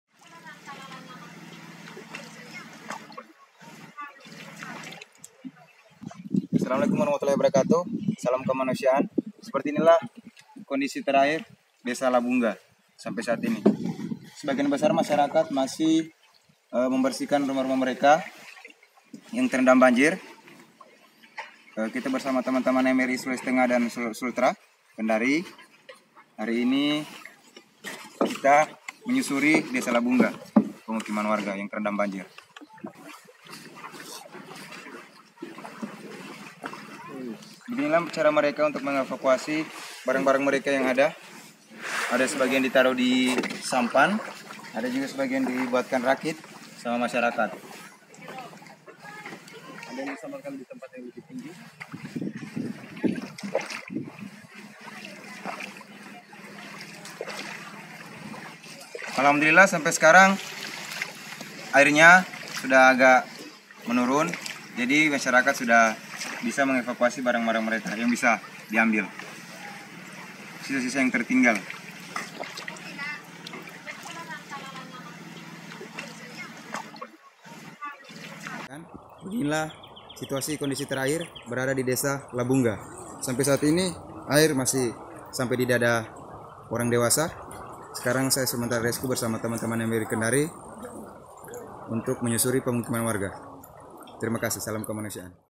Kondisi Terkini Banjir di Desa Labungga, Konawe Utara
Hingga Senin (17/6) banjir di Desa Labungga, Konawe Utara masih setinggi dada orang dewasa.

Tim Emergency Response Aksi Cepat Tanggap menuju Desa Labungga untuk membantu evakuasi warga. Sebagian besar warga masih bertahan di rumah-rumah. Menyimpan barang-barang mereka di atas sampan.